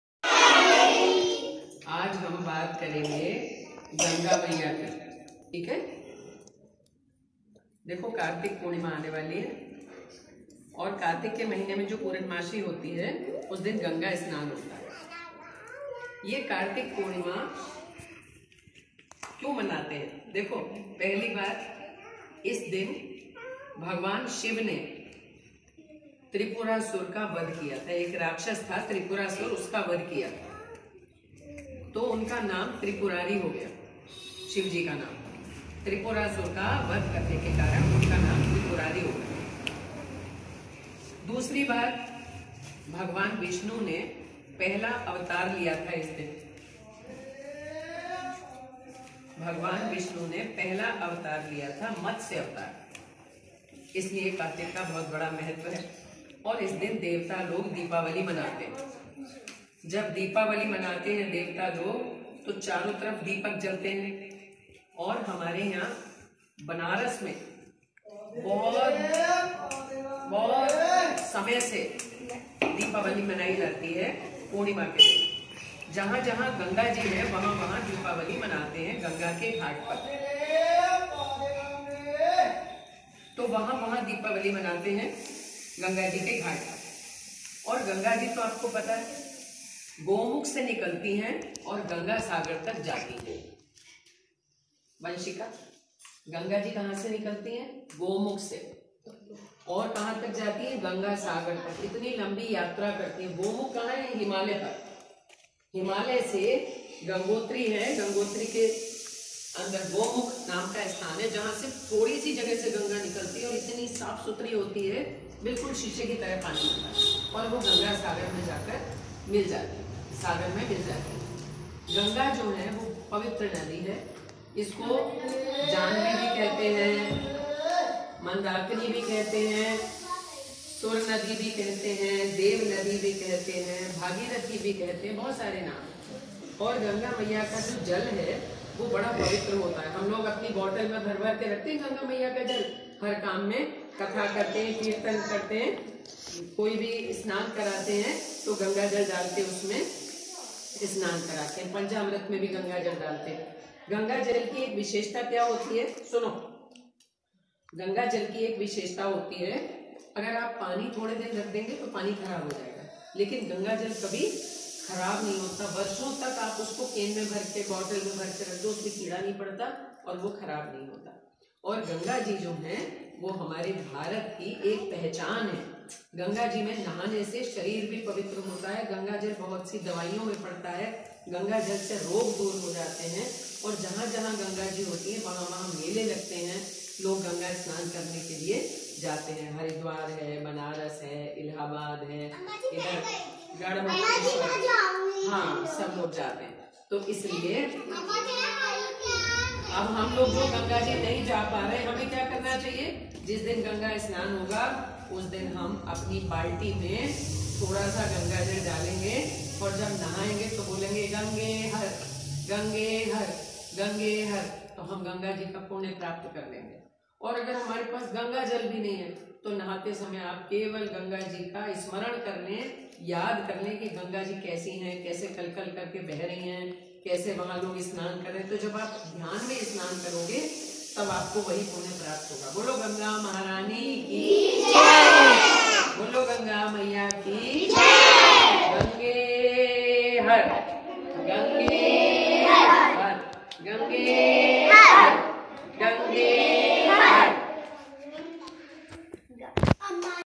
गंगास्नान,कार्तिक पूर्णिमा,श्रीमति अंजुशर्मा,बालसत्संग संस्कारशाला,श्री राधाकृष्णमंदिर फरीदपुर(बरेली
आज की कथा : कार्तिक पूर्णिमा - गंगा स्नान
बाल सत्संग - संस्कारशाला, श्री राधाकृष्ण मंदिर, साहूकारा, फरीदपुर (बरेली)

गंगा जी का नाम सुनते ही मन में शीतलता के साथ ही साथ पवित्रता का स्मरण होता है l

क्या है कार्तिक पूर्णिमा?
क्या महत्व है?
क्यूँ हमारे संस्कार एवम् संस्कृति में कार्तिक पूर्णिमा की तिथि इतनी महत्वपूर्ण है?
क्या आप जानते हैं कि त्रिपुरासुर नामक दानव का वध किसने किया?
भगवान् विष्णु जी के प्रथम अवतार का नाम क्या था और कब प्रथम अवतार हुआ था?
क्या आप जानते हैं कि देवता कब दीवाली मनाते हैं?
क्या आपको पता है कि गंगा मैया को किन किन नामों से जाना जाता है?
किस तिथि पर अधिकांश गंगा घाटों पर दीपक जलाकर दीवाली मनाते हैं?
गंगा जी का उद्गम कहाँ से हुआ और कहां जाकर सागर में मिल जाती हैं?
गंगाजल की विशेषता क्या है और कैसे वो अन्य जलों से विशेष है?

सवाल इतने सारे कि गूगल पर खोजने में भी अच्छा खासा समय लगे और जवाब सही भी हैं या नहीं इसकी कोई गारंटी नहीं l लेकिन हमारी पूज्य माँ श्रीमति Anju Sharma जी की संस्कारशाला में होने वाले बाल सत्संग के सभी नन्हें मुन्ने सक्रिय सदस्य (सोशल मीडिया के माध्यम से संस्कारशाला का बाल सत्संग एवम् इसके सभी सदस्यों की प्रतिभा अब देश- विदेश के हजारों-लाखों लोगों तक पहुंचता है) इन सभी प्रश्नों का उत्तर भली भांति जानते और समझते हैं l और यहां हम आपके लिए और आपके परिवार विशेषकर बच्चों के लिए संस्कारशाला के आज के बाल सत्संग को सिर्फ 4 मिनट लगभग में समेट लाए हैं और वो भी एक मीठी कहानी की तरह l

आओ, सुनो - सुनाओ और "गंगे हर" "गंगे हर" का जयकारा लगाओ l

हर हर महादेव, जै गंगा मैया, ओम नमो नारायणाय